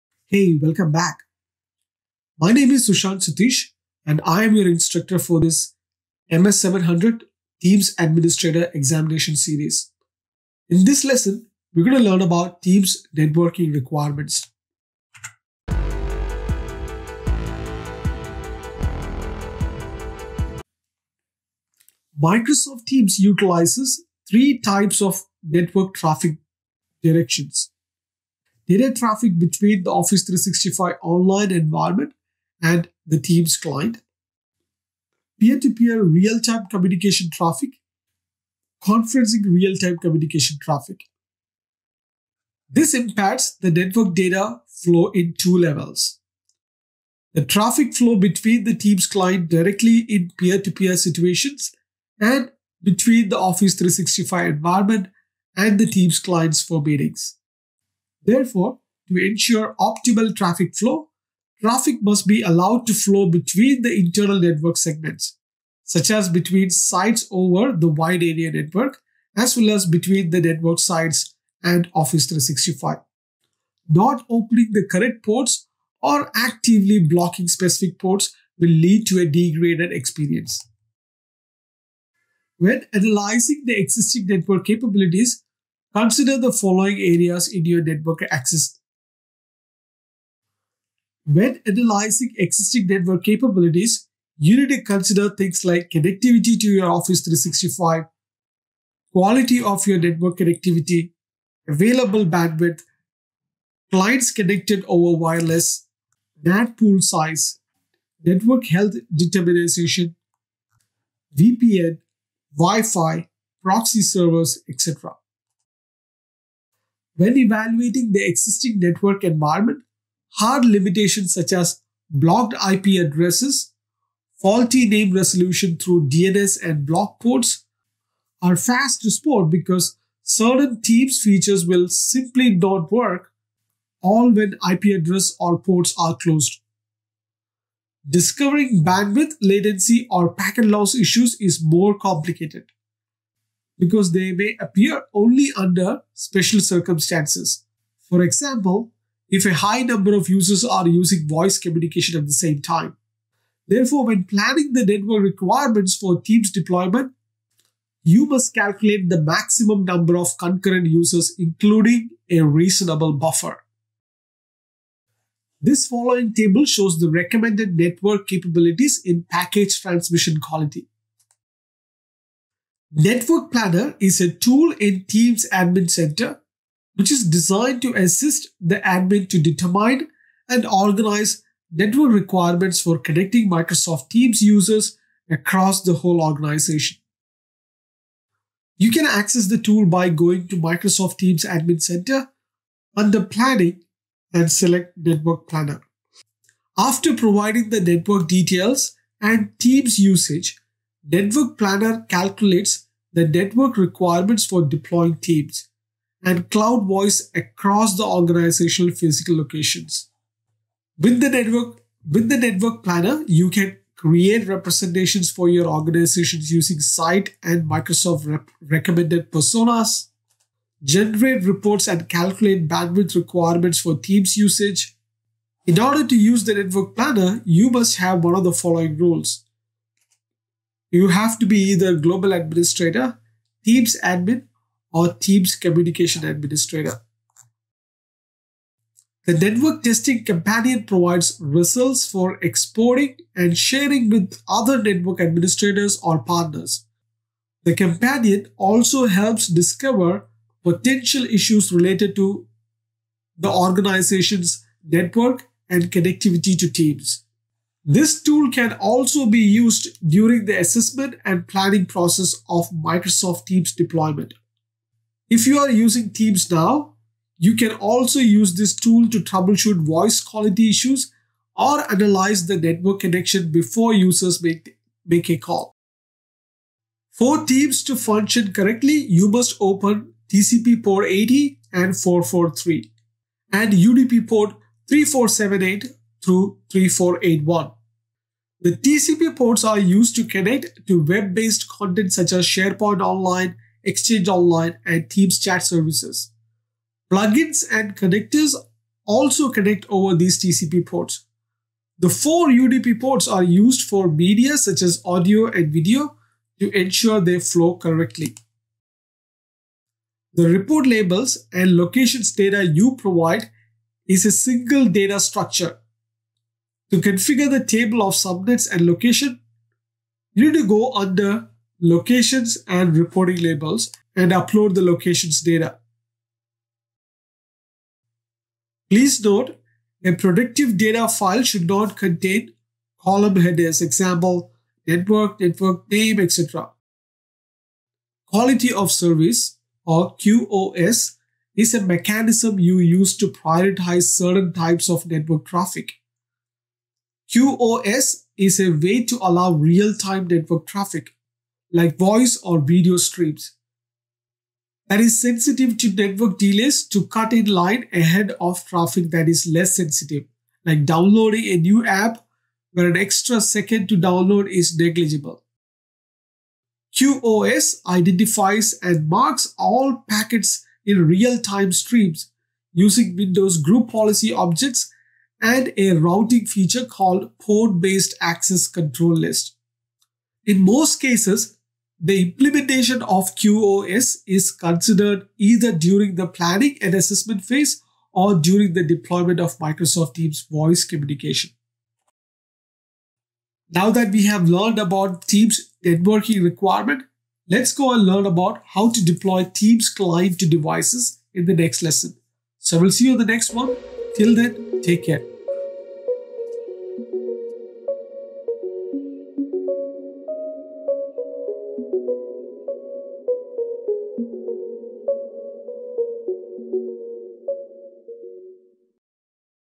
MS-700 Exam EP 43: MS Teams Networking Requirements
⏰ Timestamps:

00:00​​ ⏩ Introduction
00:21 ⏩ Overview Of Teams Networking Requirements
01:21​​ ⏩ Considerations For Networking Capabilities
01:56 ⏩ Plan For Your Network Requirements
02:51​​ ⏩ Use The Network Planner
03:58​ ⏩ Use The Network Testing Companion
04:35​​ ⏩ Configure Network Ports And Protocols
05:09​ ⏩ Configure Reporting Labels For Microsoft Teams
05:37​​ ⏩ Implement Quality Of Service (QoS)
06:32​​ ⏩ What's Next